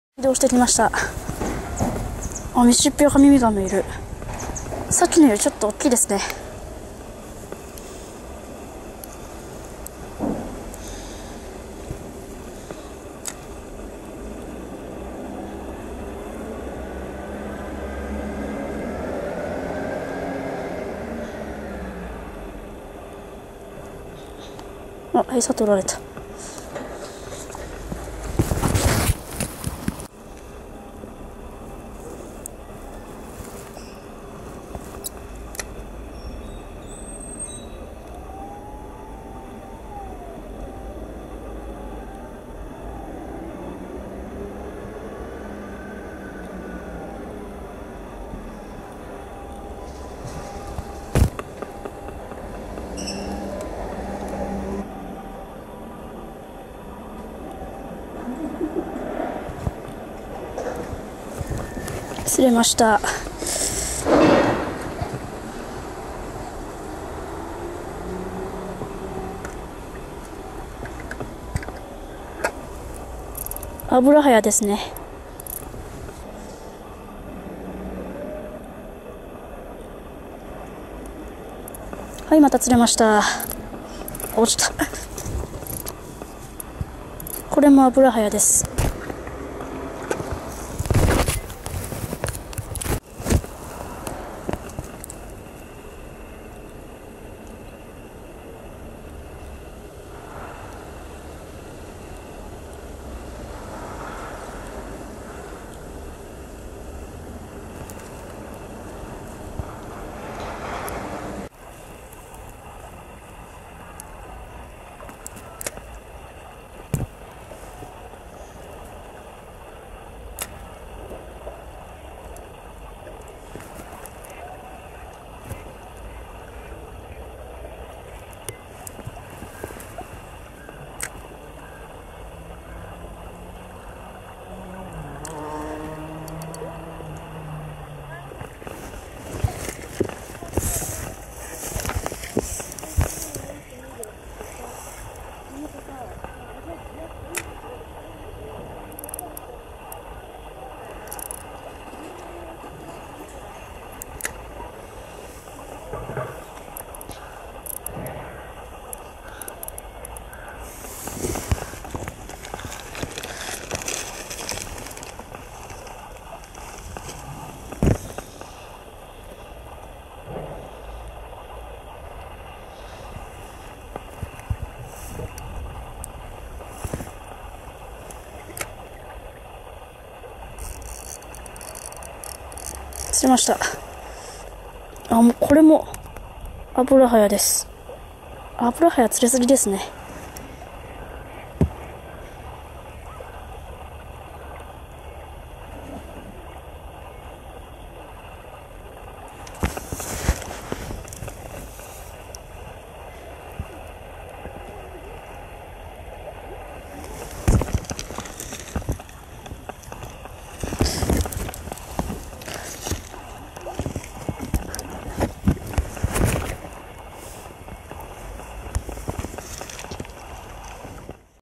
コンパクトロッドで小物釣り 黒目川 いつもより上流編 黒目川にやばい外来種も
黒目川で餌釣り。使用タックル　オリンペッド２１０　リール　セドナ　餌　フィッシュワゲット　針　特選たなご１号